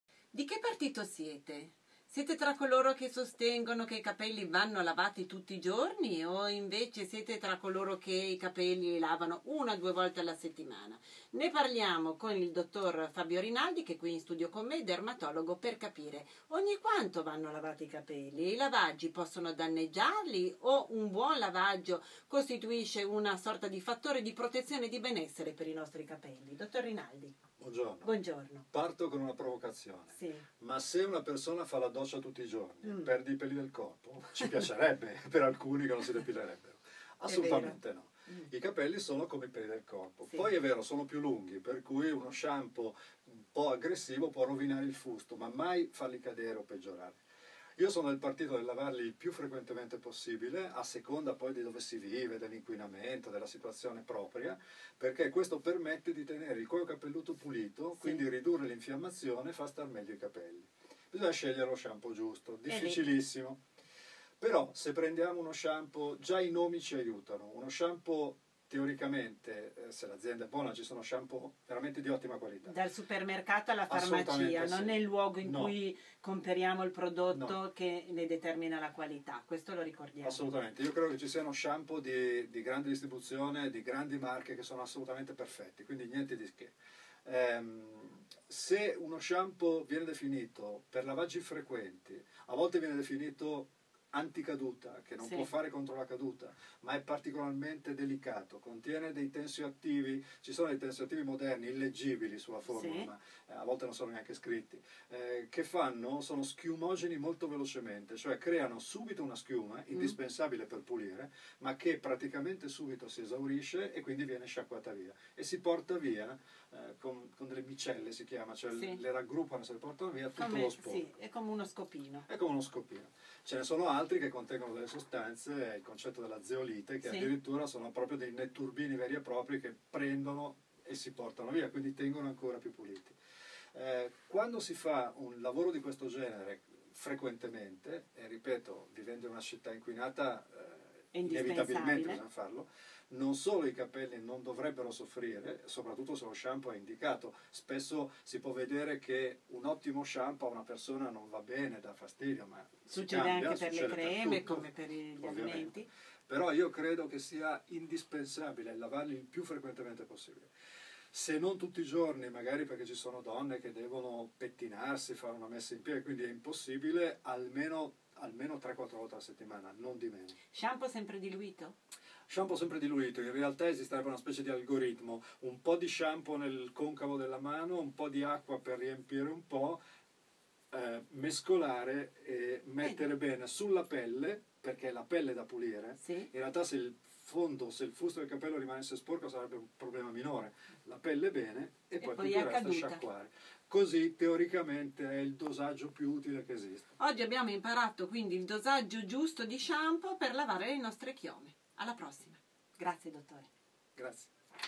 Capelli: come e quanto lavarli
Nicoletta Carbone intervista il dr. Fabio Rinaldi, Specializzato in Dermatologia e Venereologia presso l'Università di Milano e presidente della fondazione internazionale di ricerca tricologica